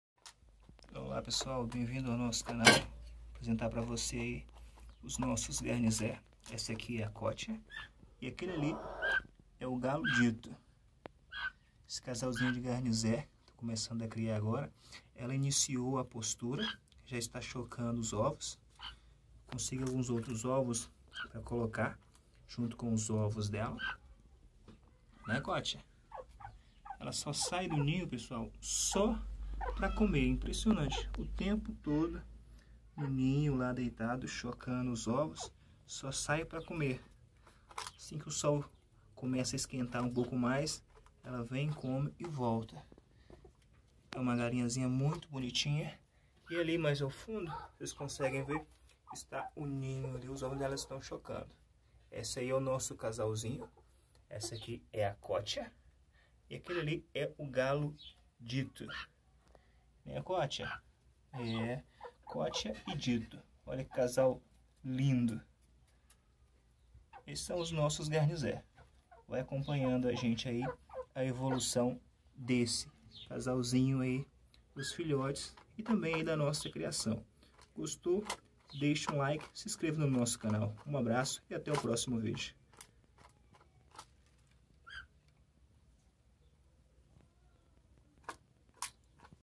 Meu casal de Garnize.
Minha pequenina galinha esta chocando pela primeira vez.